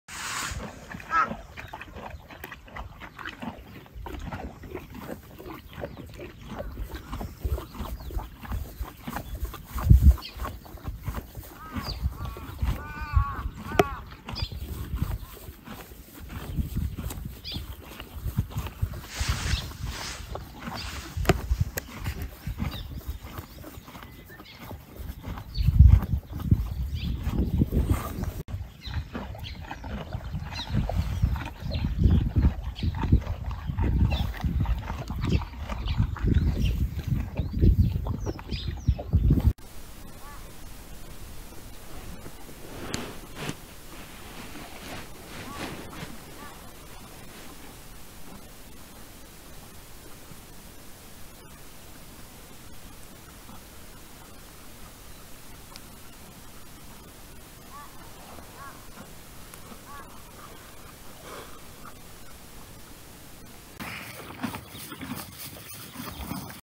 青草を美味しそうに食べるリカコ
小さなパドックのため、生えている青草はほぼ無くなりましたが、カットした牧草をもらって嬉しそうに食べています。
2023年5月1日　沖田牧場さん分場にて